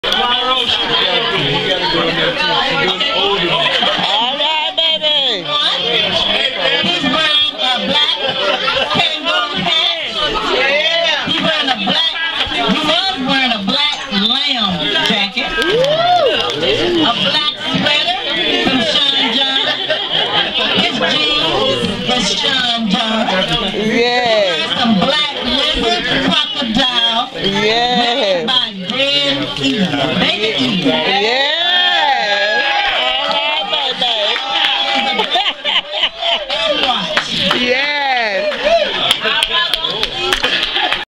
MONROE CHRISTMAS 2015 "MEN FASHION SHOW" WILLIAM "BAYBAY"
Monroe family christmas a yearly event filled with love, family, friends, food and fun. But mainly gathered for the reason reason behind Christmans...JESUS IS THE REASON.